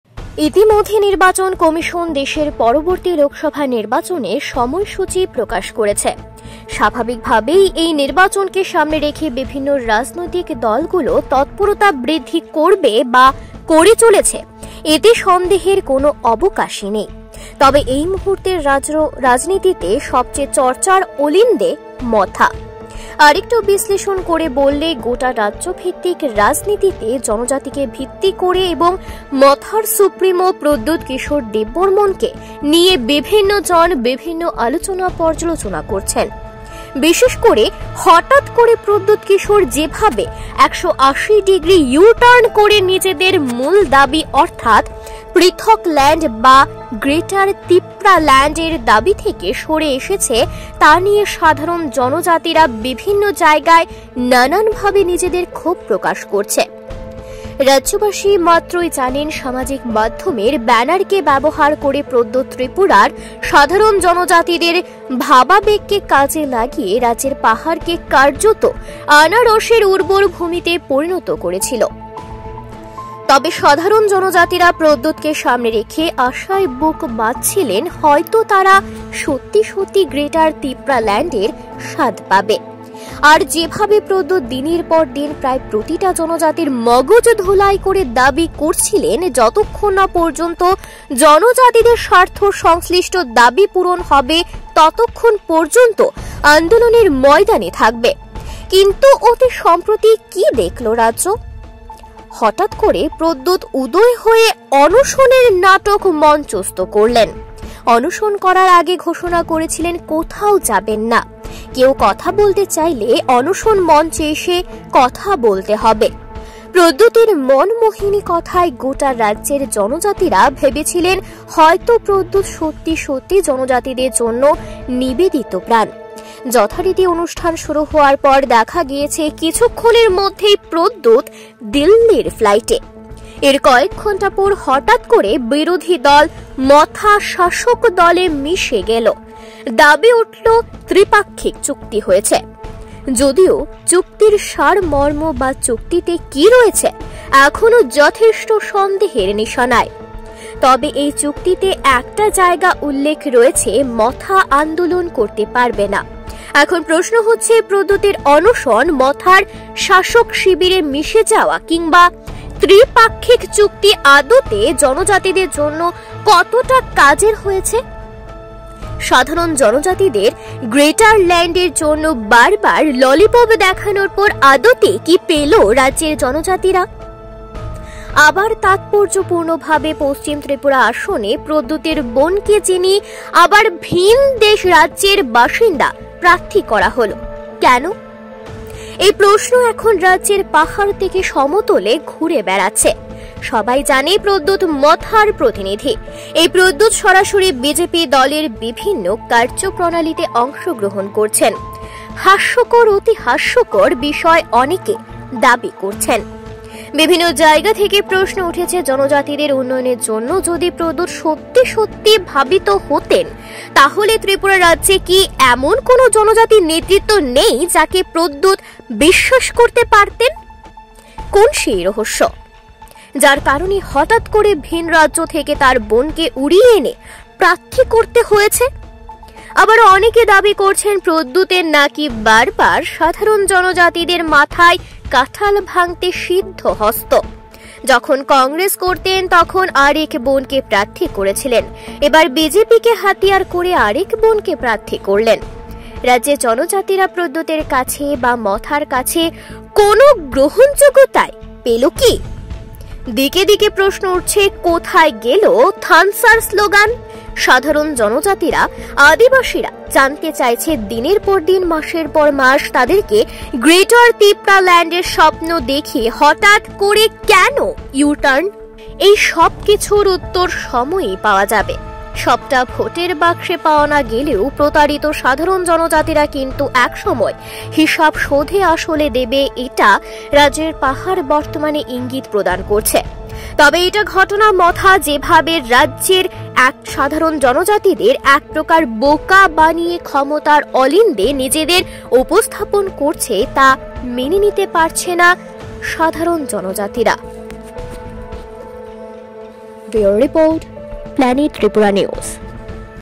মথার ডিগবাজিতে বিরূপ প্রতিক্রিয়া রাজ্য জুড়ে | Tipra Motha Party News | Planet Tripura
Audio Credits:-
Planet Tripura News